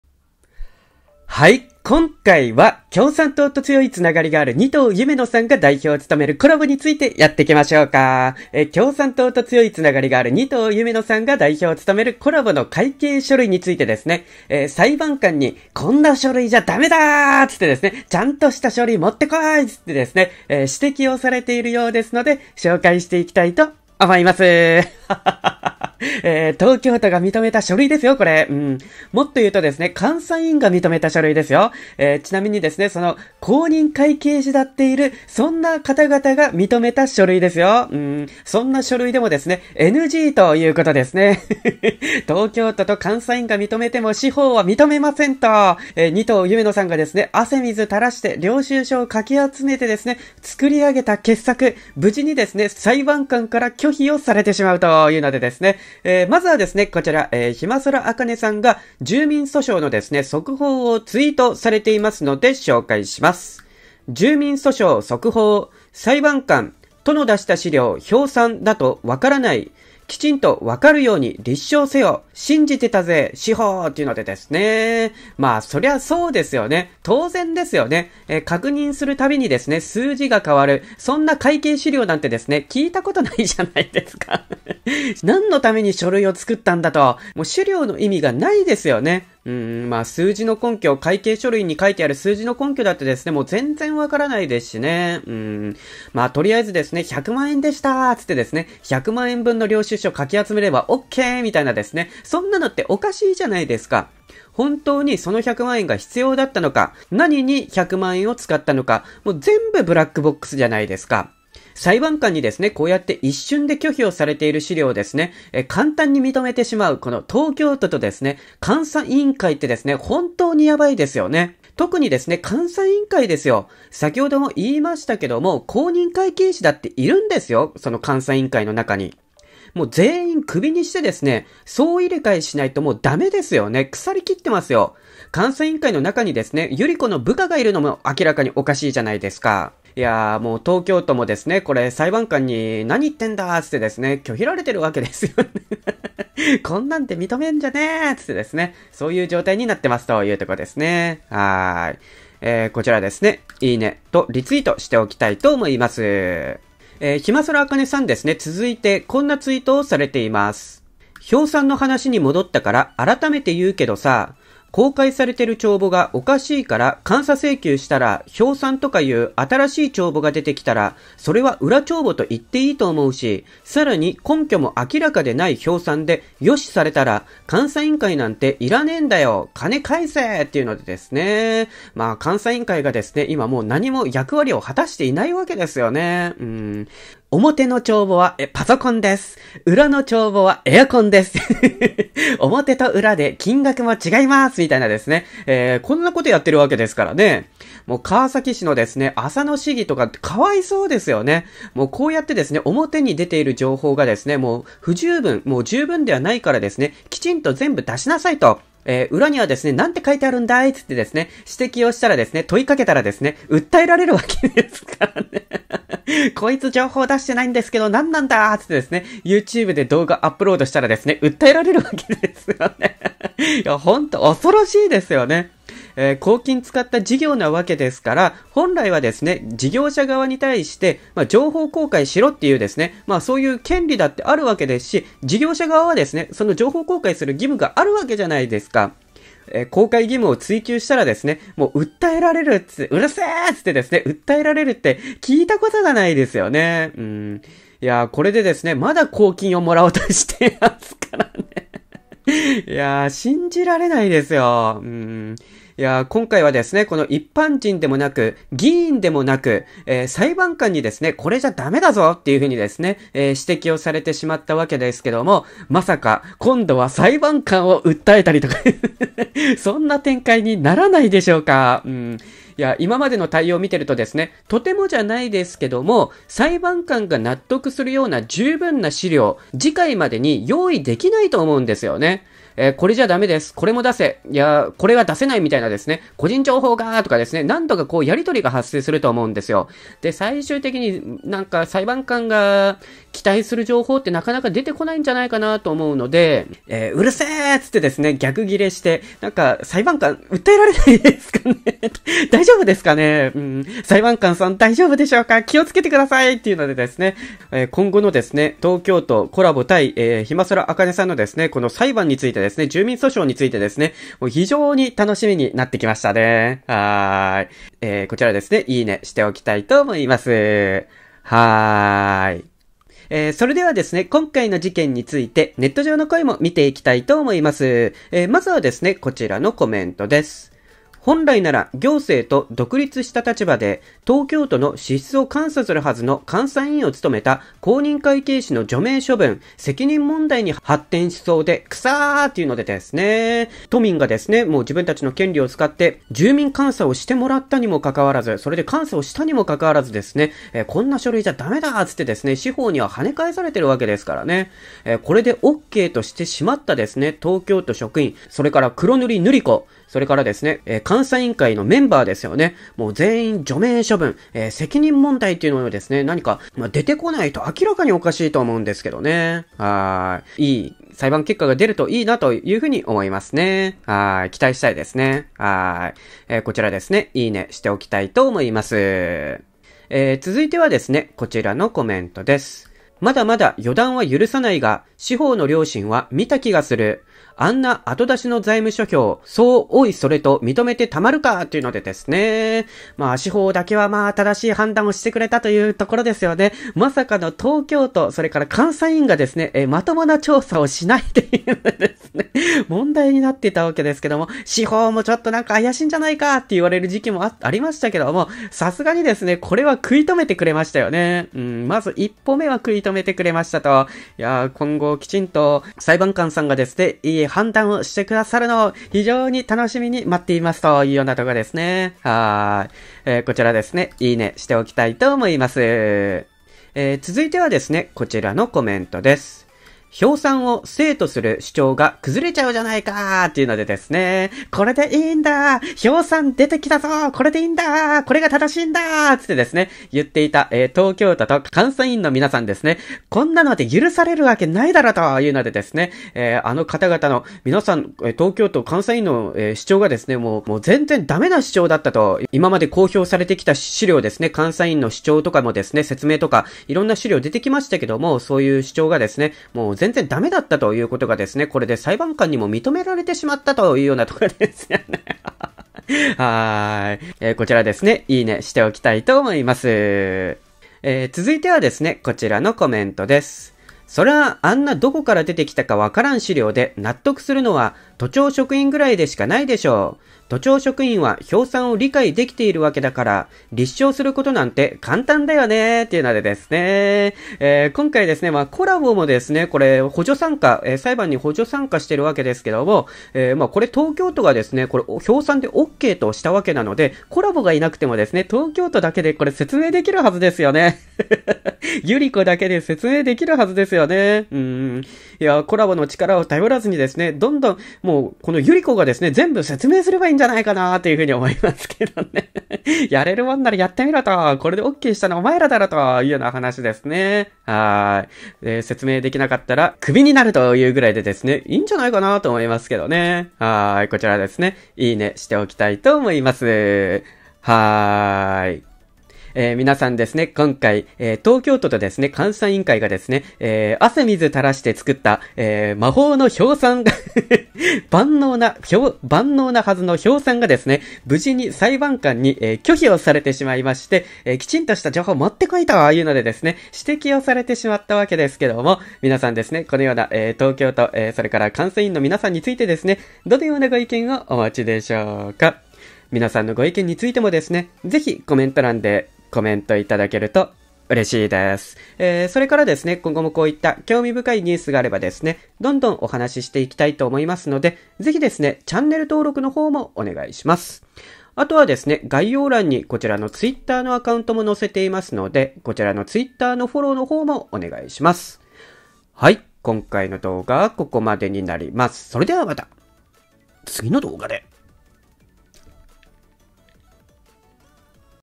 Colabo仁藤夢乃、魔法の表3会計書類、裁判官は認めずwww【Masaニュース雑談】
撮影・編集：MacBookPro M2 2022